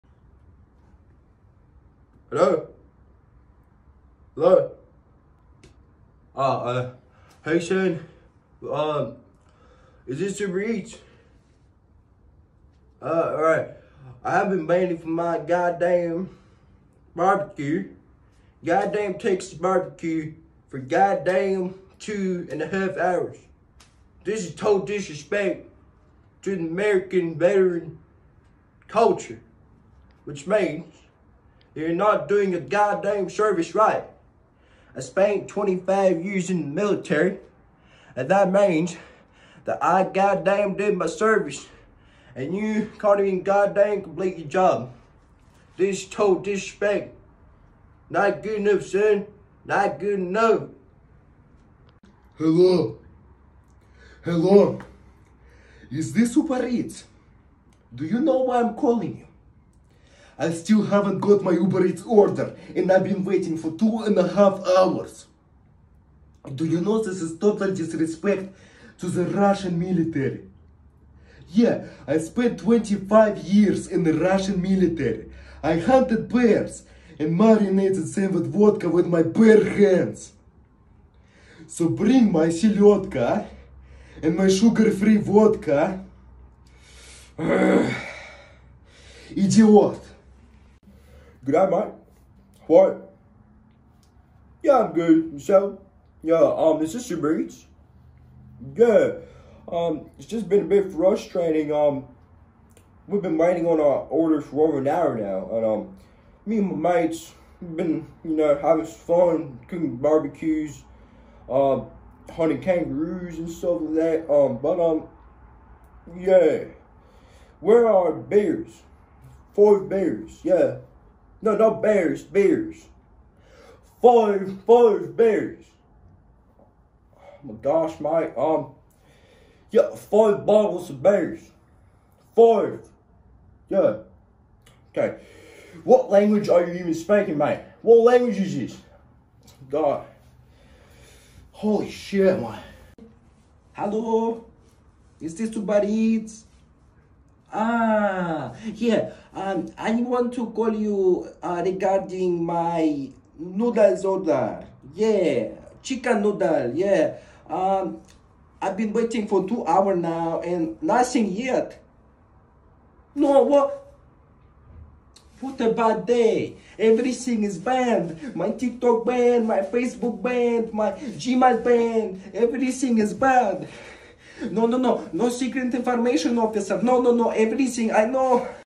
When veterans around the world call Uber eats (USA, Russia, Australia and China)
First video guys! Have a great day! More videos coming!

And TikTok @antonthelagunov